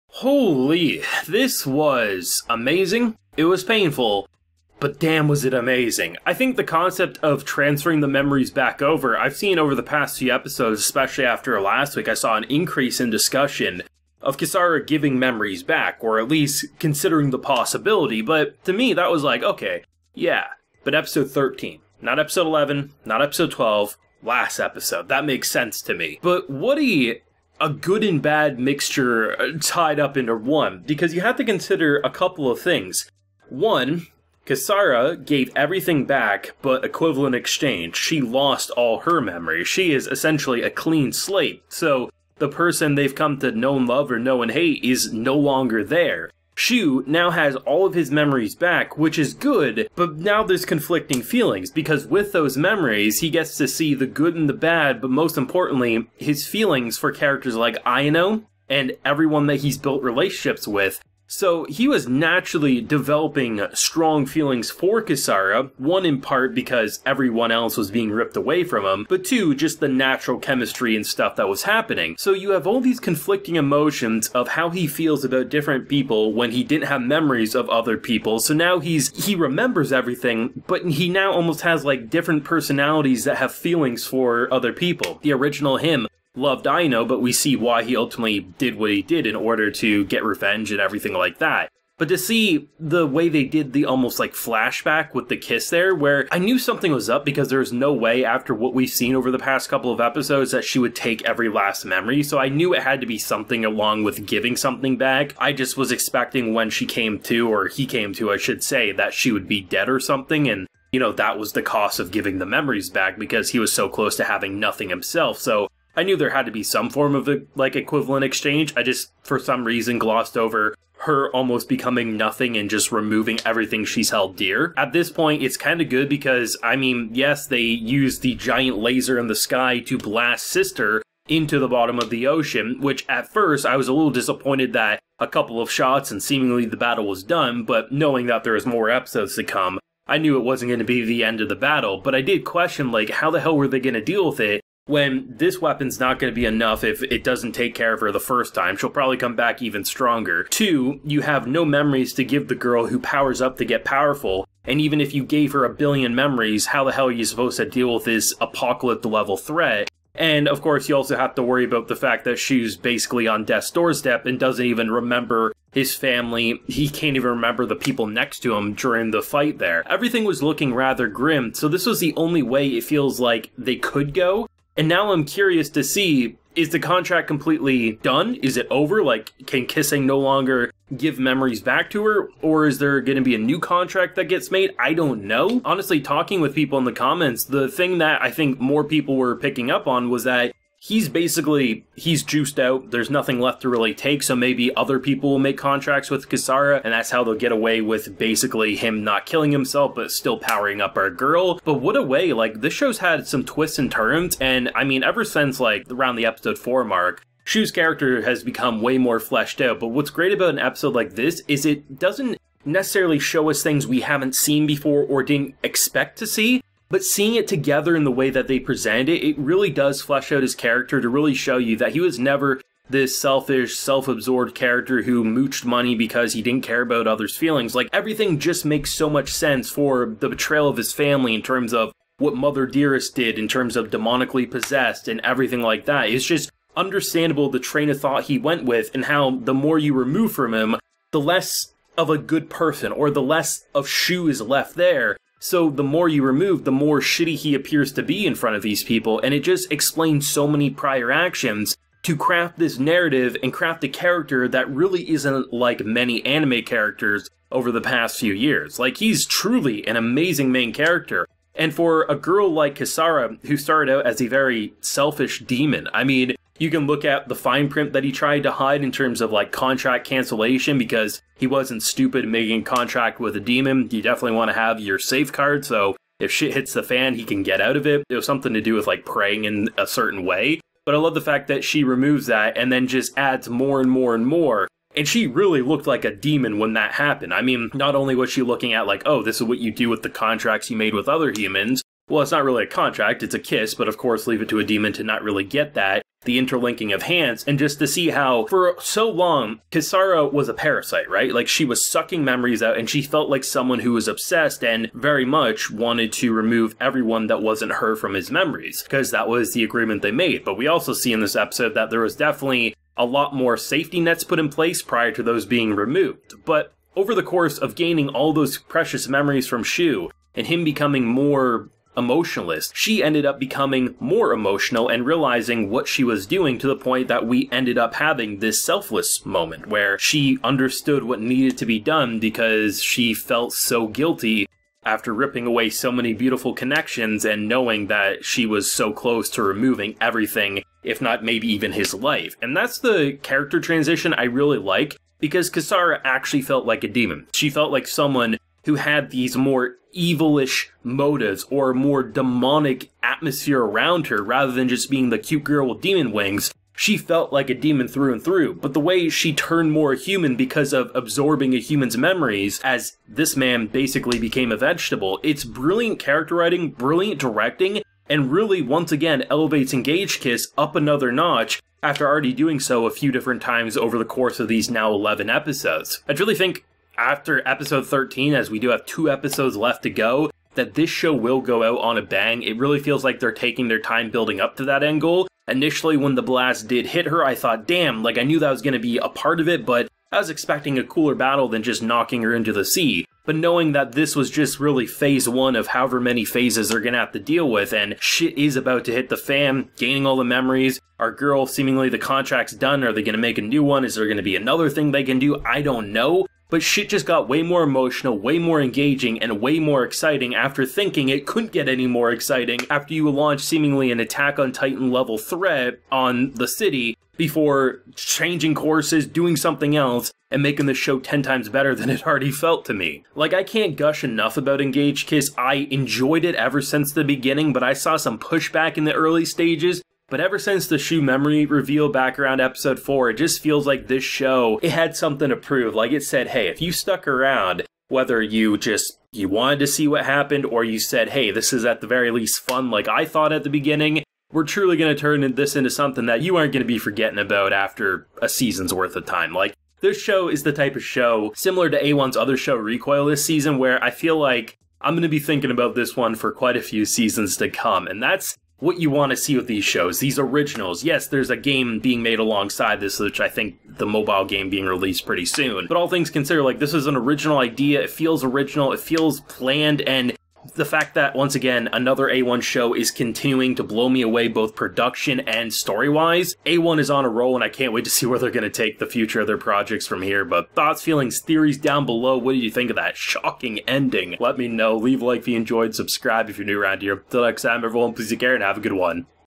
FROM SELFISH DEMON 😈 TO SELFLESS HERO 😟 | Engage Kiss Episode 11 Review
Kisara like Shu has had an amazing character arc so to see where she started to her current actions truly is night and day.

✦ Calvin Atkinson ✦ Teeny bopper ✦ Sign ✦ Vu Nguyen ✦ Ferno19z ✦ sweetrae21 ✦ Kevin Nauta ✦ James ✦ Harry Truscott ✦ Didrik Svahn ✦ sai wentum